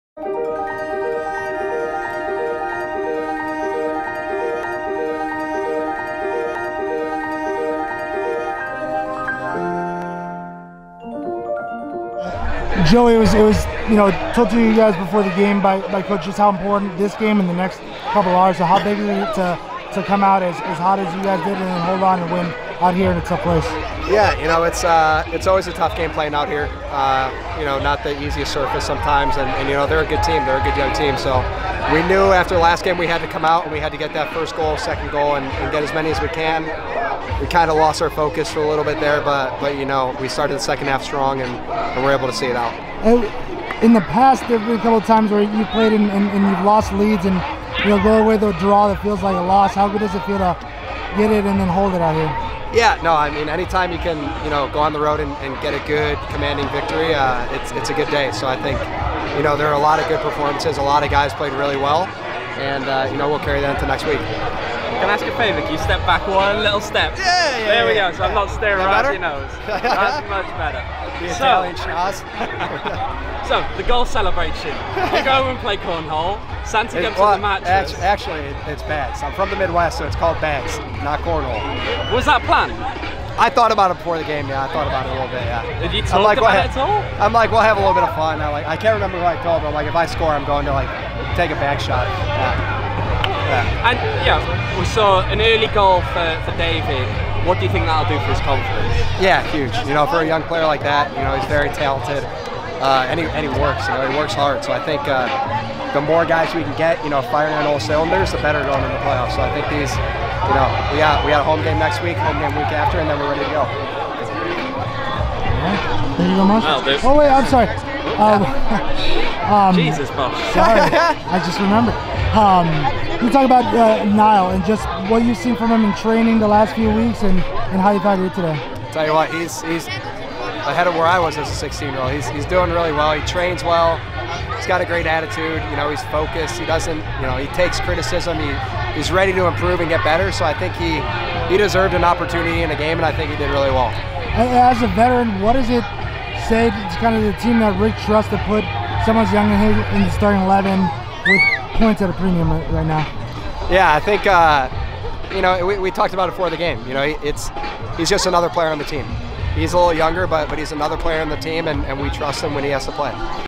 Mayo Clinic Postgame: Joey Calistri
Midfielder Joey Calistri speaks to media following the club's dominant 6-3 win over Las Vegas.